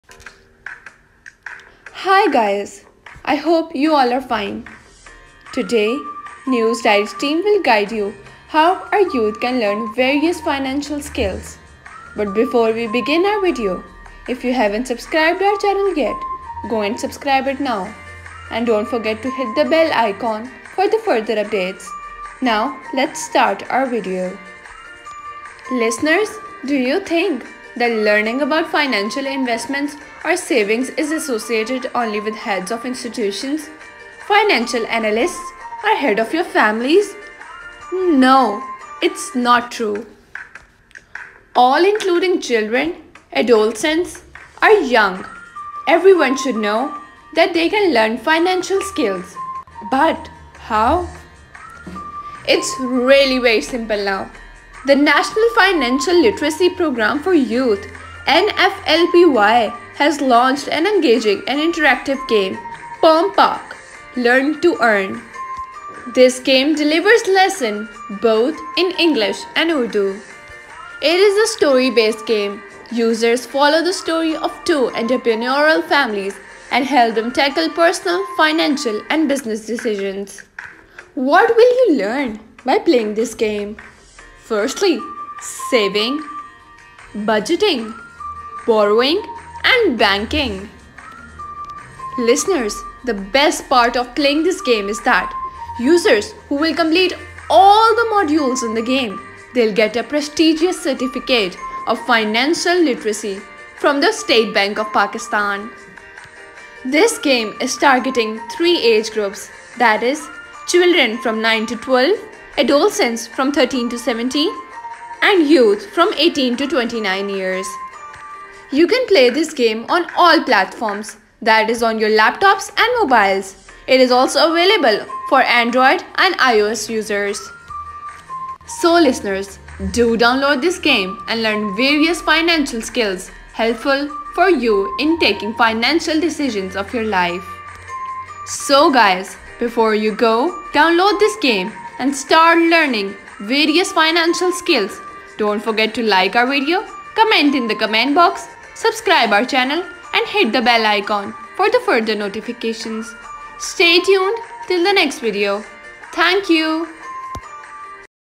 POMPAK— Learn to Earn
This is a gaming app launched by NFLP-Y which can help you learn various financial skills in a fun way.